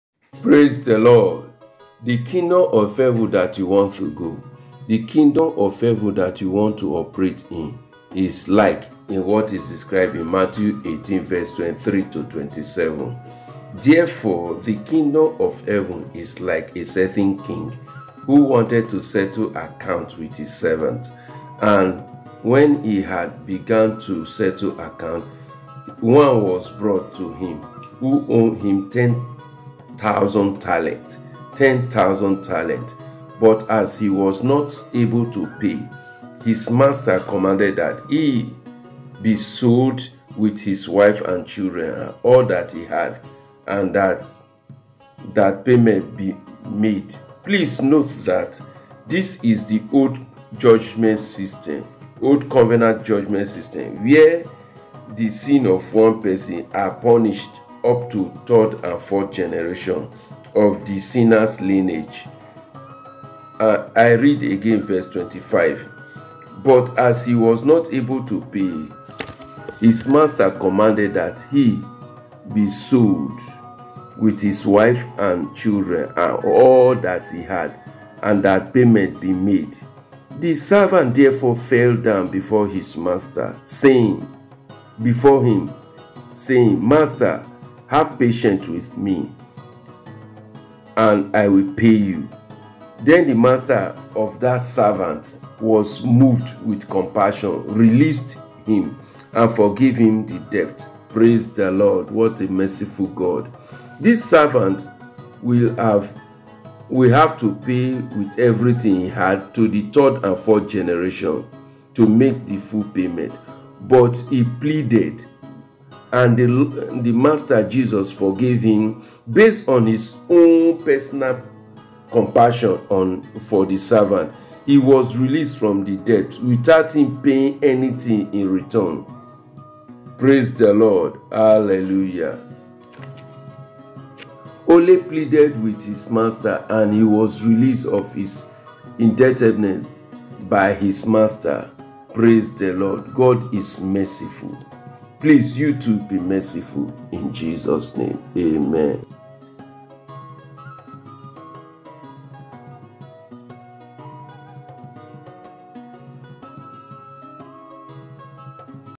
How To: Of Forgiveness 2. #020
This edition is the extract of The Faith of The Son of God Part 5C which is a continuation of our previous work (The Faith of The Son of God Series) bringing more light to the foundations, we have been laying on the applications of ancient truth. These are ancient truth that are base on certain principles.

As we said earlier, they are methods of charting your life and destiny to properly enter into your God’s predestination, even if it has been derailed by you or by the society or your enemies.

They are base on ancient secrets that has been tested by eternal fire seven times, hence it cannot fail.

They are in series, broken into bits, for easy comprehension for application by all age brackets.

God bless you as you watch them.

For any inquiries, suggestions or questions, please leave it in the comment box below and I will answer them.

 Stay blessed until our next edition.

For pictures:-